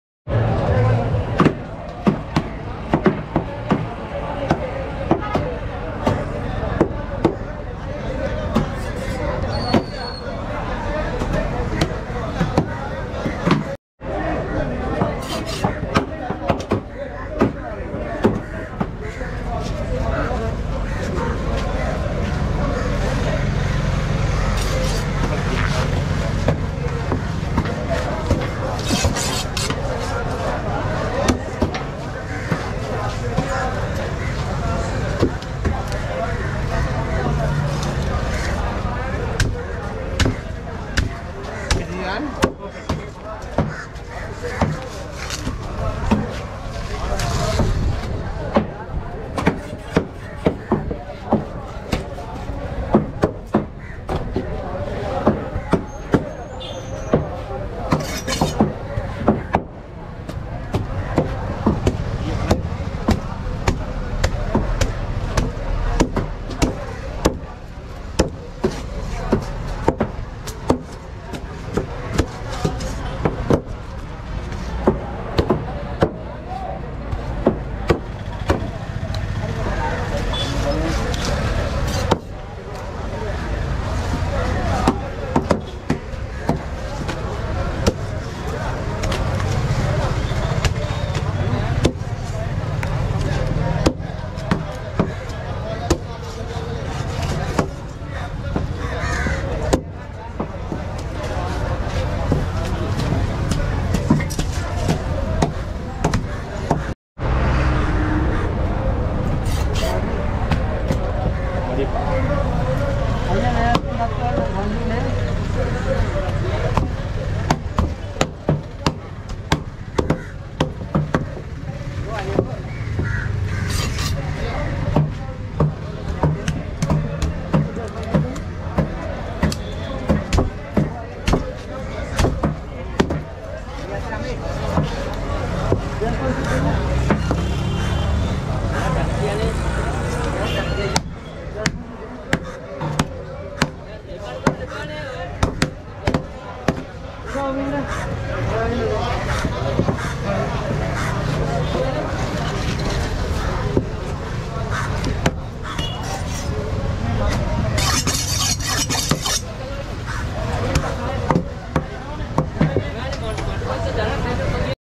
HUGE Diamond Trevally | fish cutting sri lanka | නරල්ලා
Cutter Name : Mr. NIMAL

Fish  ditls 👇

Name             : DIAMOND TREVALLY FISH

Local Name : NARALLA (නරල්ලා)

Weight           : 9.3kg

price                : $3.7(1kg)